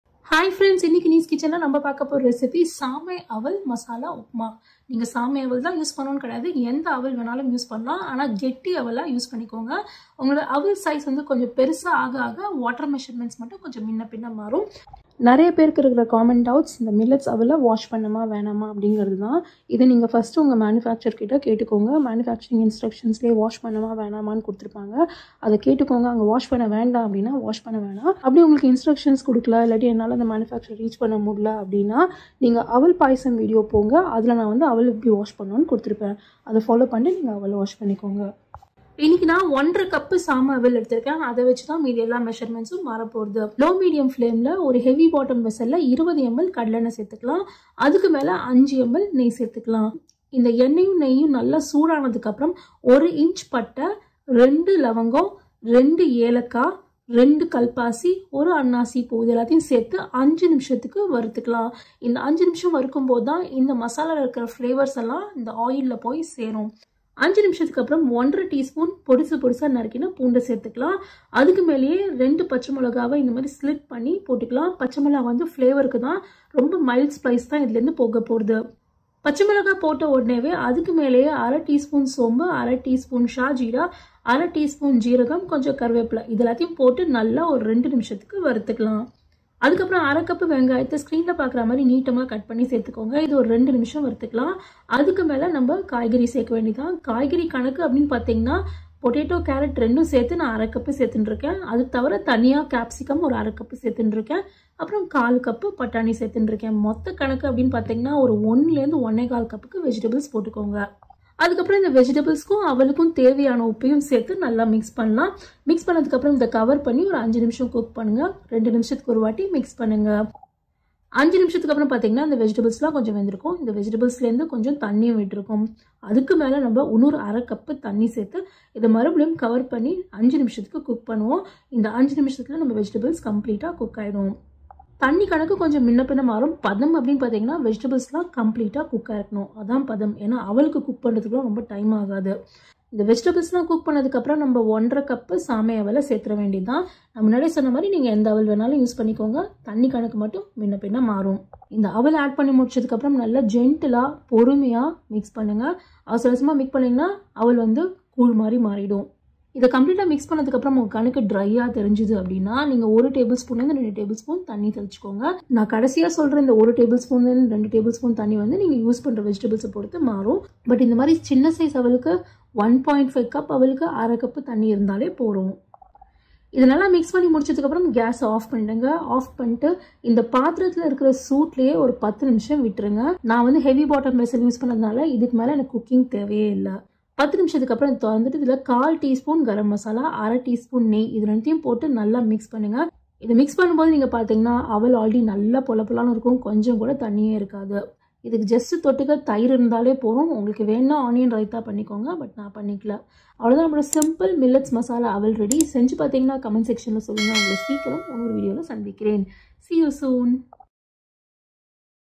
Millet flakes Pulav | Millet flakes masala Upma
Masala Aval
## Ingredients
Little Millet Flakes – 1.5 cups (Any thick variety millet flakes can be used)
Onion – 1/2 cup (Thinly sliced)
Vegetables – 1 – 1.25 cups (Chopped potato & carrot – 1/2 cup; chopped capsicum – 1/2 cup, peas – 1/4 cup)
Water – 1/2 cup (+ 1 – 2 tbsp)
## Tempering
Peanut Oil - 20 ml
Ghee – 5 ml (+ 1/2 tsp) Cinnamon Stick – 1 inch
Clove – 2
Cardamom Pod – 2
Black Stone Flower – 2
Ginger – 1.5 tsp (Finely chopped) Green Chillies – 2 (Slit)
Caraway Seeds – 1/2 tsp
Salt – As Required (Per taste) Garam Masala – 1/4 tsp